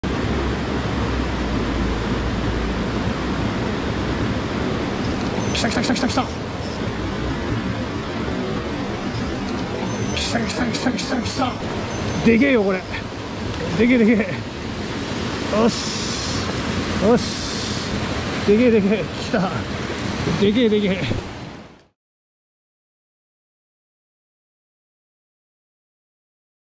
DAISO Minnow - dollar store lures
I caught a trout with a 100-yen shop lure.